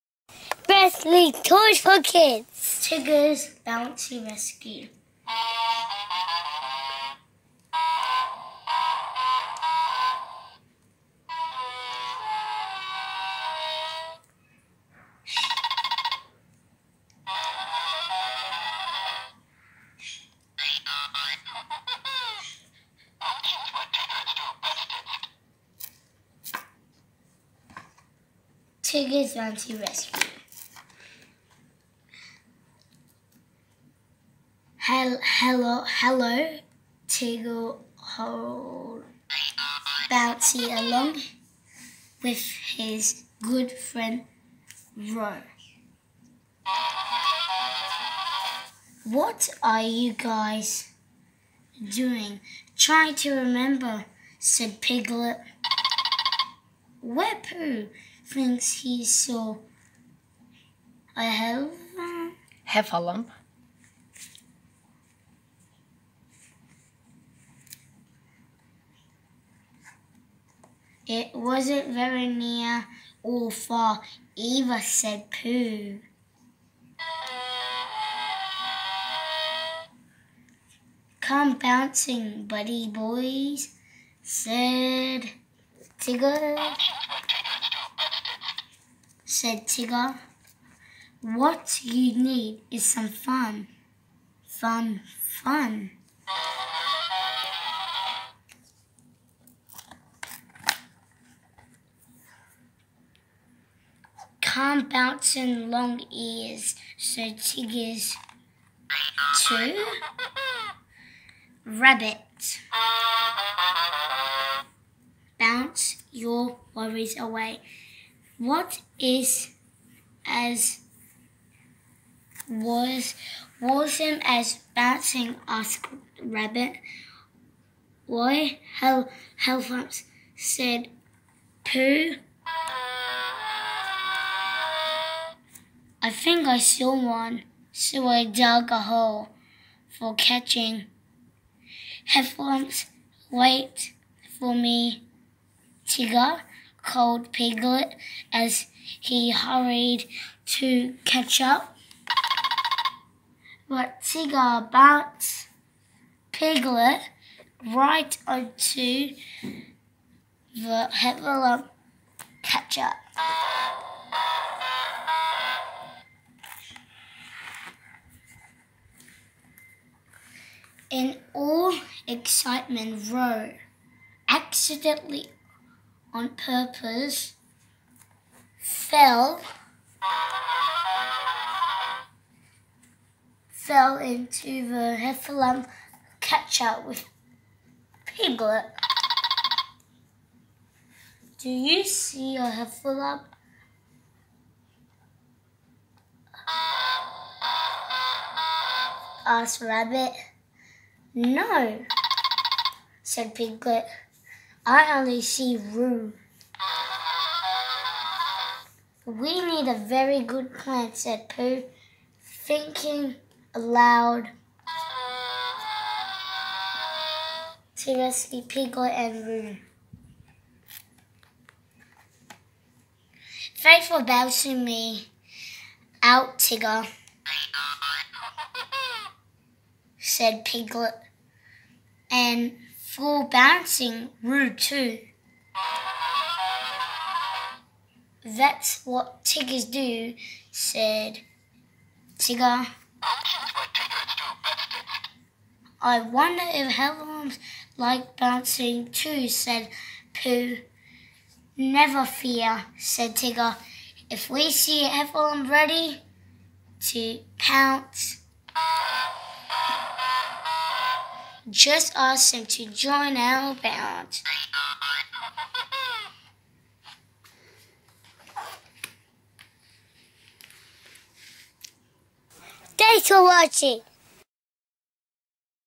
Disney Tigger's Bouncy Rescue Play-a-Sound Book | Read Aloud For Children
Presley reads Tigger's Bouncy Rescue book, I hope you enjoy my video.

Book by Kelli Kaufmann